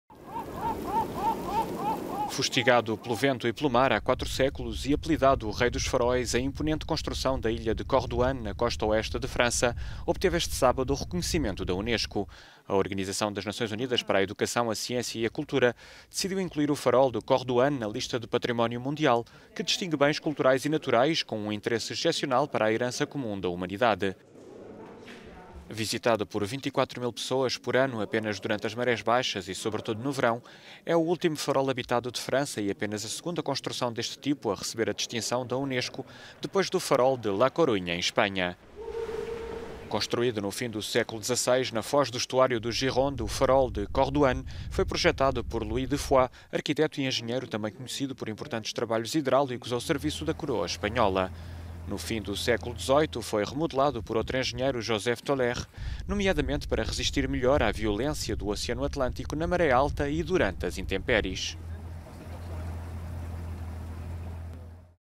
Farol de Cordouan é património mundial da UNESCO
"Rei dos Faróis" é a última construção deste tipo habitada em França